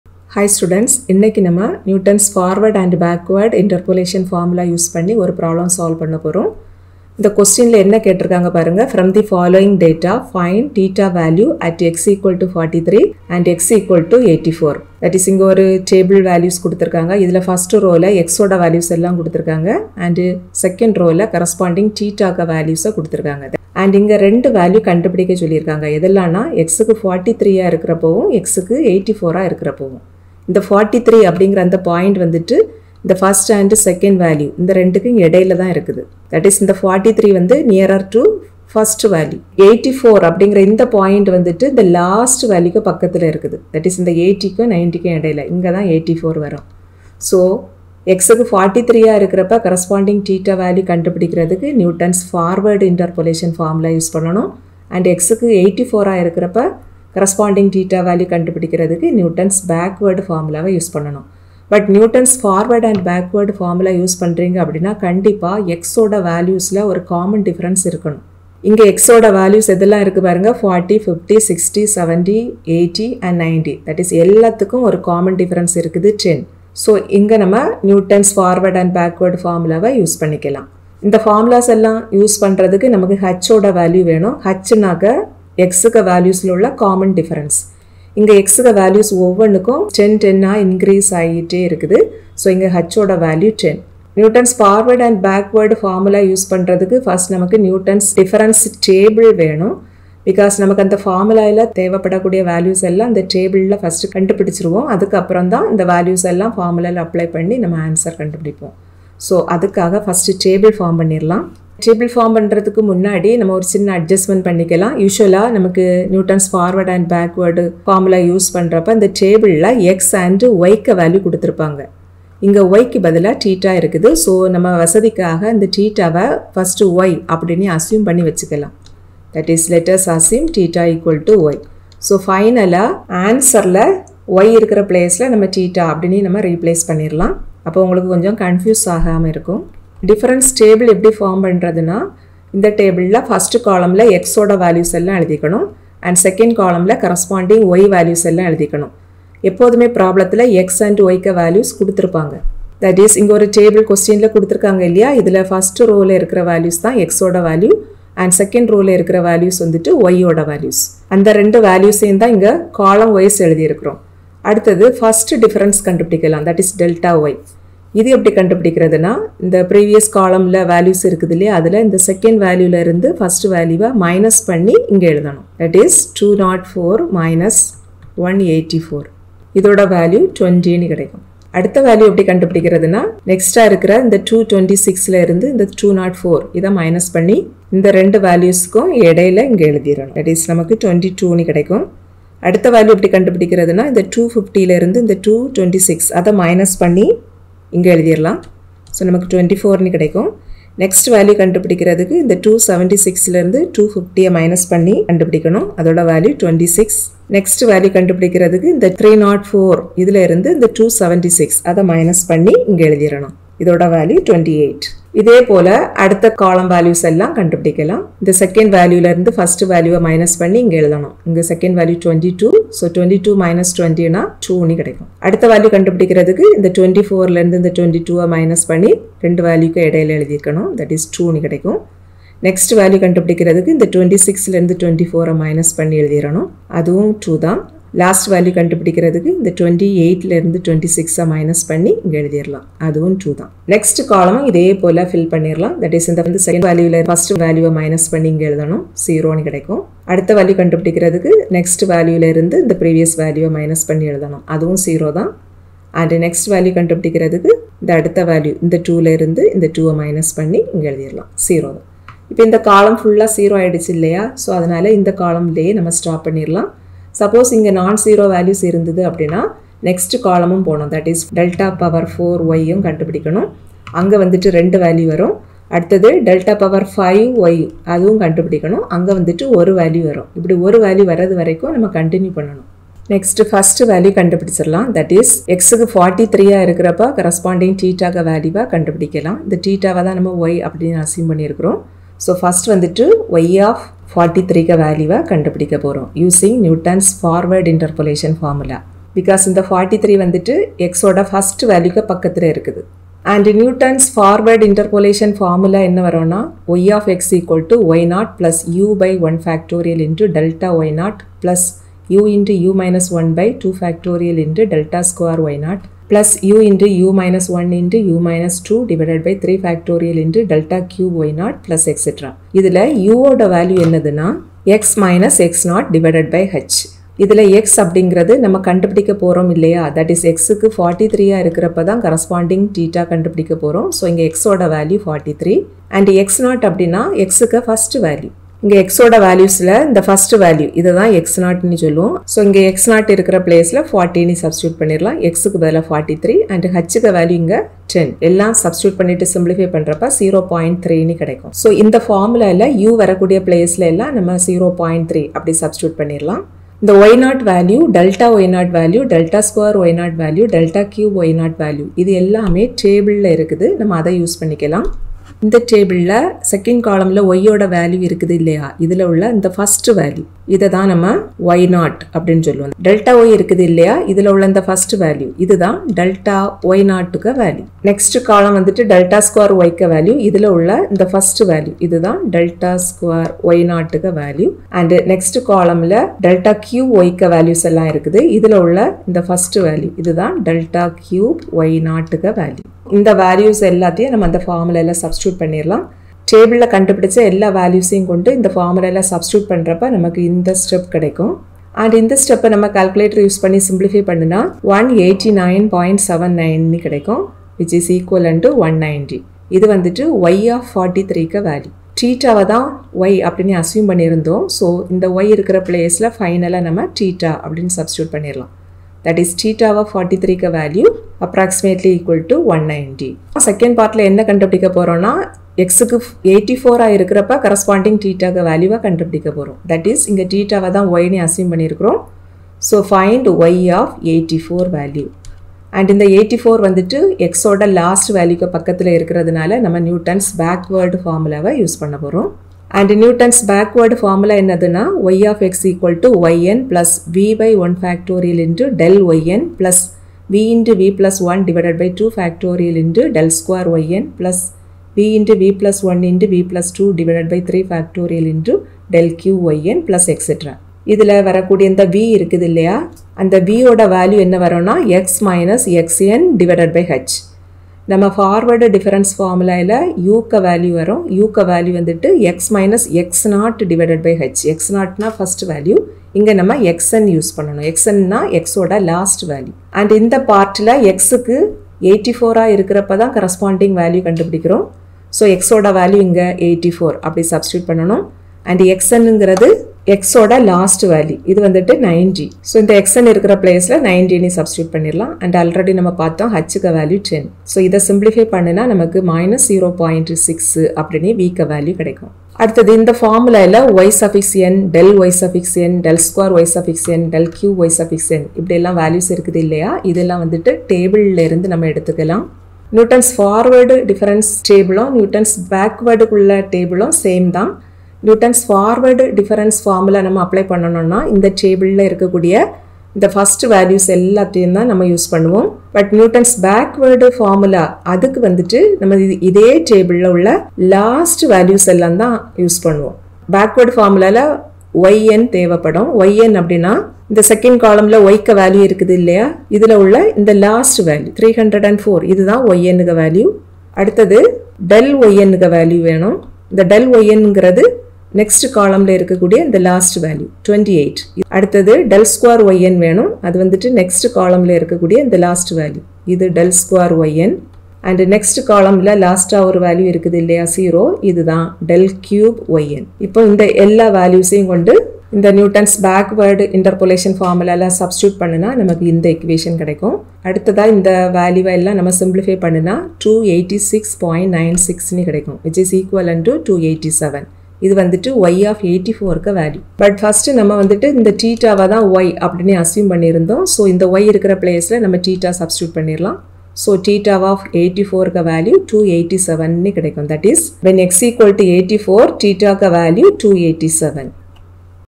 SNM | MA3251 | Unit 4 |Interpolation|Newton's forward & backward difference formula|Problem in Tamil
SNM | MA3251 | Unit 4 | Numerical Differentiation | Newton's forward  & Backward difference | Problem in Tamil

MA4151- APPLIED PROBABILITY AND STATISTICS FOR COMPUTER SCIENCE ENGINEERS